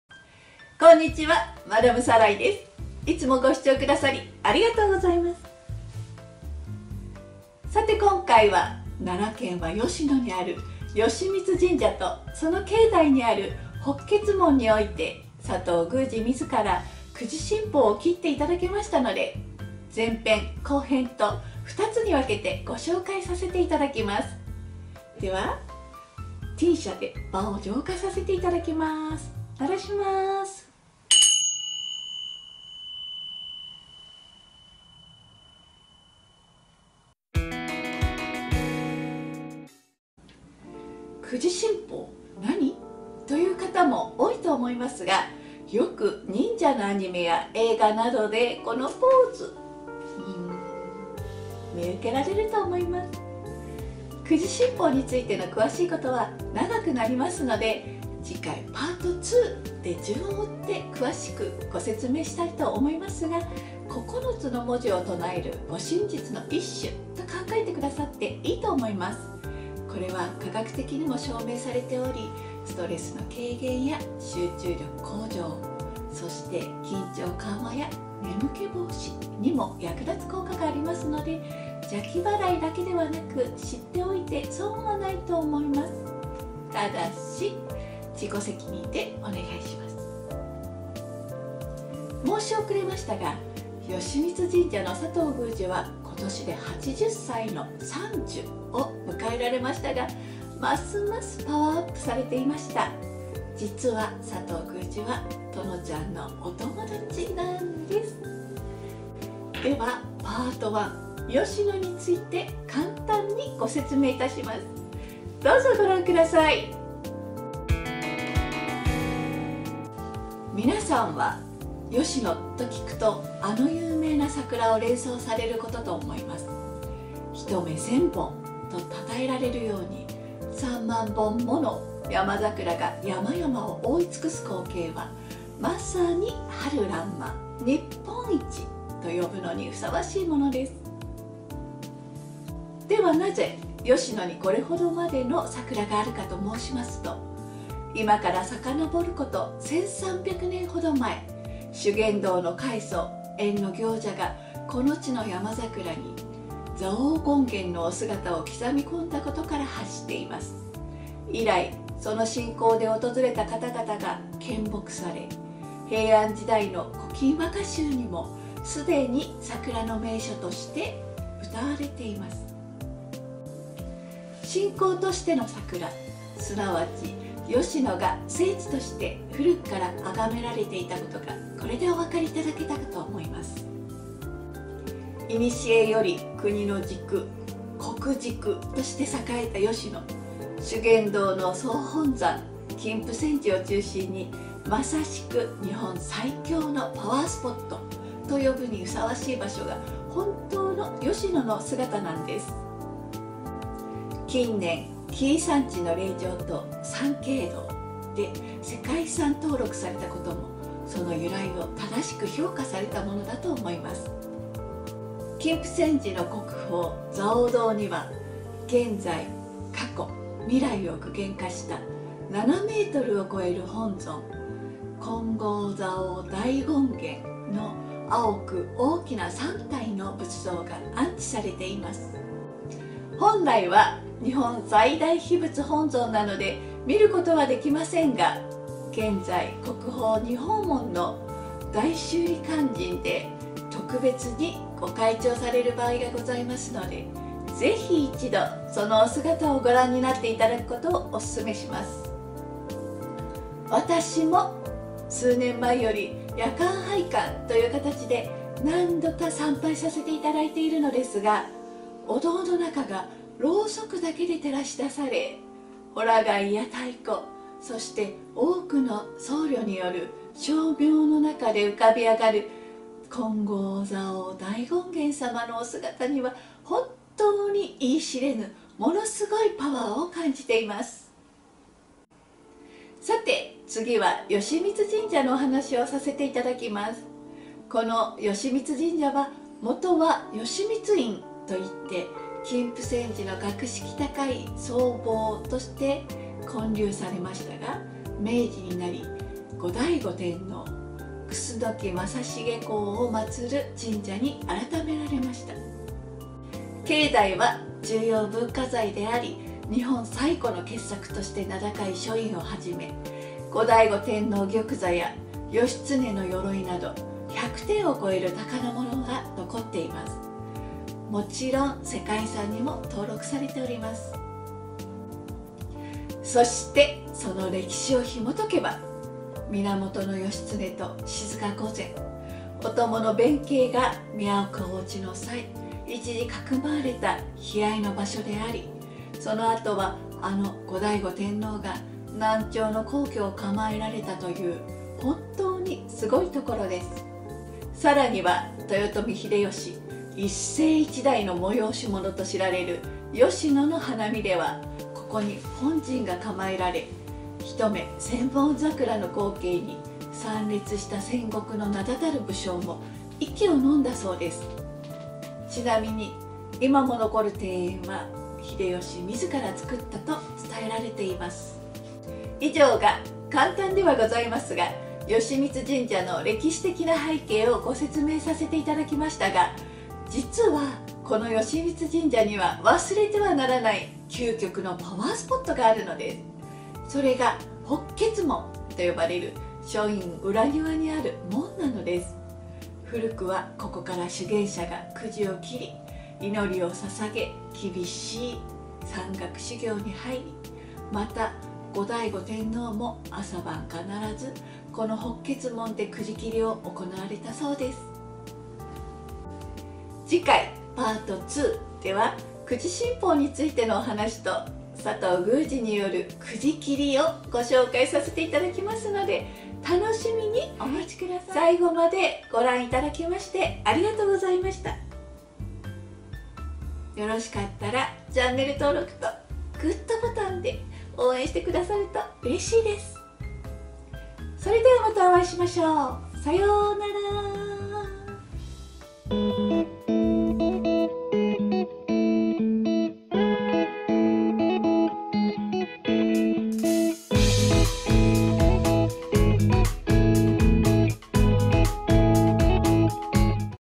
【吉野 吉水神社 金峯山寺】1300年前から日本最強のパワースポットなんです！
日本最強のパワースポット「吉野」日本一の桜のルーツから、聖地としての吉野を掘下げてみました。そして、映画やアニメでお馴染みの「九字切り」を、そのメッカともいえる吉水神社北闕門にて佐藤宮司自ら切って戴けましたのでご紹介致します。

Part1では、吉野の聖地の要たる修験道総本山・金峯山寺と南朝皇居としても有名な吉水神社の歴史を中心にご紹介します。

BGM m(_ _ )m
FREE BGM　DOVA-SYNDROME

They are very wonderful artists!

イラスト　あるばさん　いらすとやさん　お世話になっています!